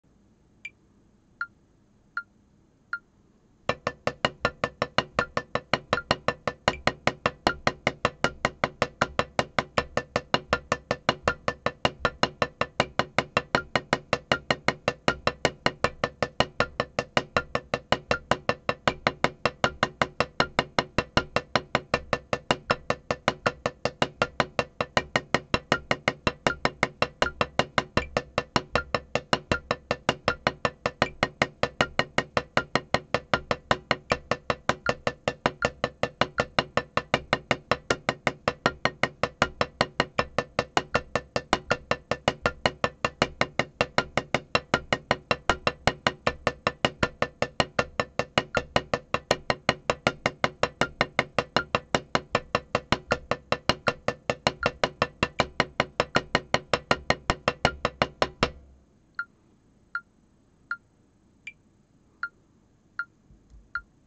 3連打の練習（♩＝79）/ 2023.08.22
手順
RRRL | RRRL | RRRL | RLRR
LLLR | LLLR | LLLR | LRLL

右手と全く同じ感覚で
左手を動かせるようになりたい

不自然な動きをせず
素直に振りたい